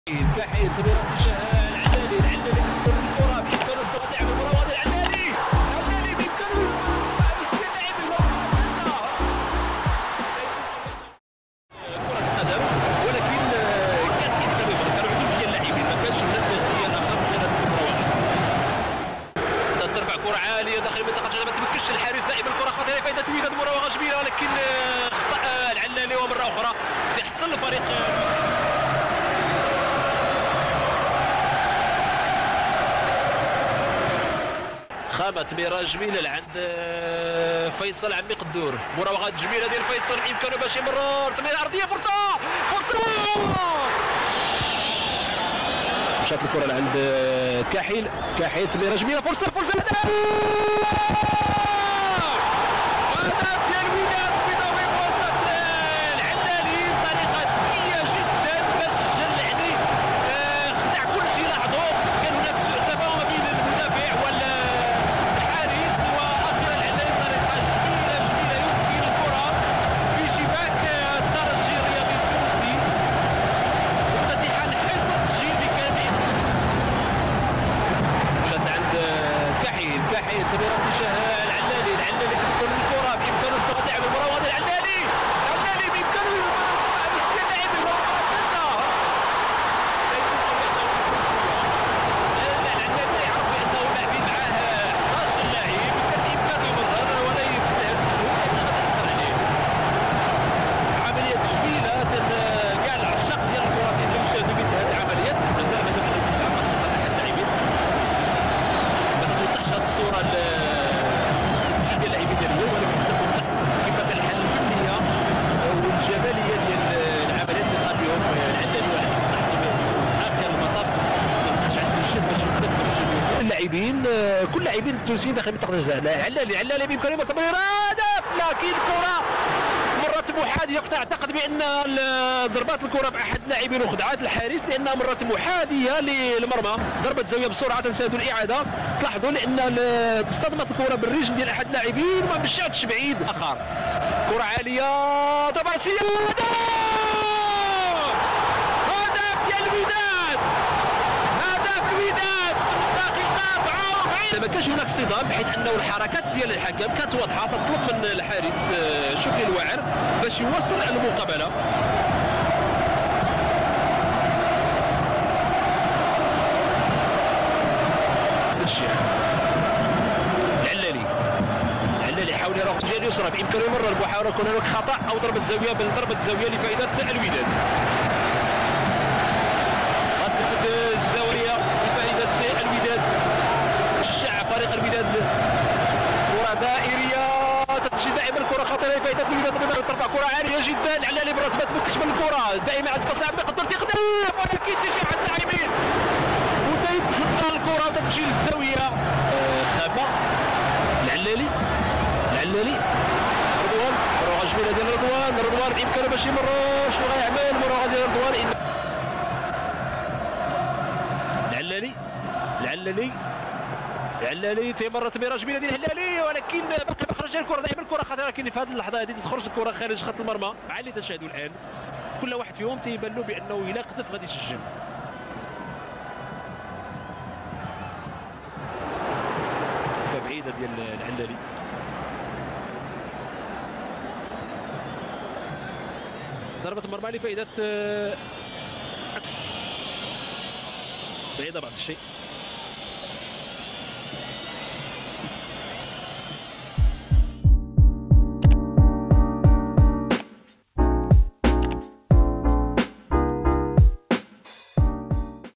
Redouane El Allali vs Esperance sportive de tunis 1998 ⚡ رضوان العلالي ضد الترجي الرياضي التونسي
1998, le Wac accueil l'équipe tunisienne du "taraji" pour le compte de la demi-finale retour de la coupe d'Afrique des vainqueurs de coupe de football.

Battue 1-4 au match aller, l'équipe wydadi avait pour objectif de battre l'ésperance par au moins 3 buts sans en encaisser pour pouvoir se qualifier en finale et affronté l'equipe angolaise Clube Desportivo Primeiro de Agosto.

Le wydad a dominé son match et a créer beaucoup de problèmes à l'équipe tunisienne, emporté par l'enthousiasme et l'envie de revanche de ses jeunes talents, le wydad finis par gagner 2 - 0, mais pas suffisant pour obtenir la qualification, du moins tous les Marocains étaient fiers de ce groupe qui a tout donné sur le terrain.

Allali, lui, a fait une performance stellaire conclue par un but, portant son équipe vers l'avant, il a également martyrisé la défense adverse par ses nombreux dribbles et percussions.

Quel Match !